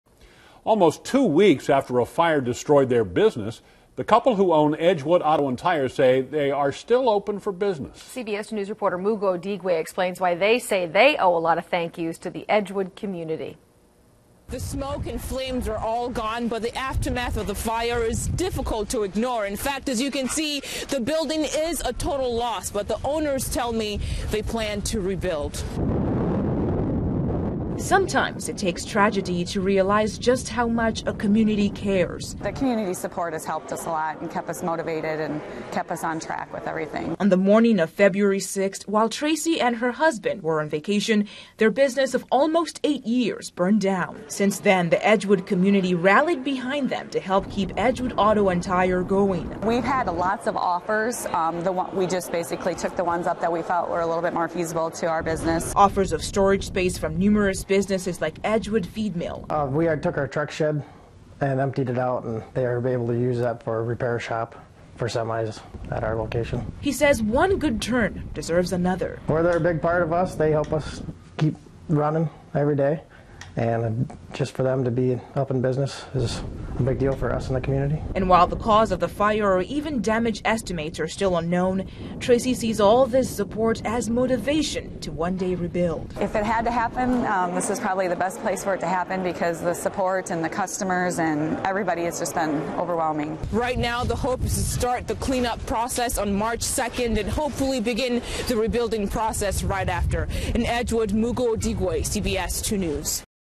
Community Helps Edgewood Business
EDGEWOOD, IA (CBS2/FOX28)--Almost two weeks after a fire destroyed their business